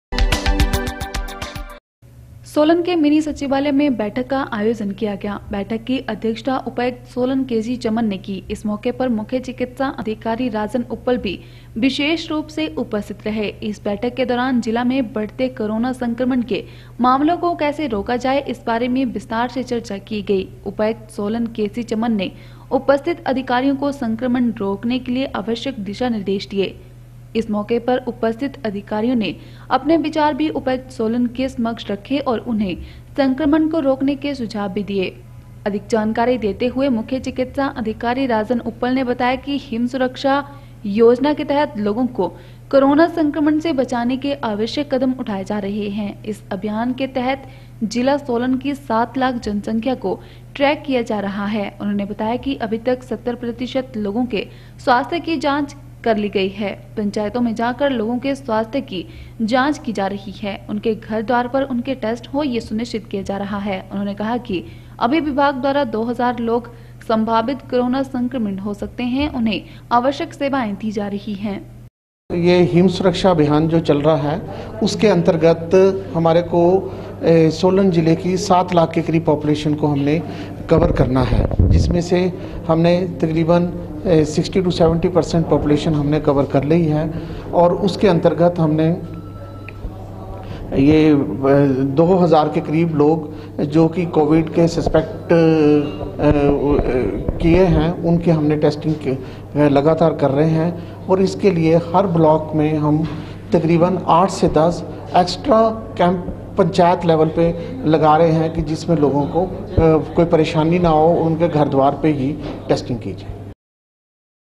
19 dec 5 अब पंचायतों में जा कर घर द्वार पर होंगे कोविड टैस्ट : राजन उप्पल
अब पंचायतों में जा कर घर द्वार पर होंगे  कोविड   टैस्ट : राजन उप्पल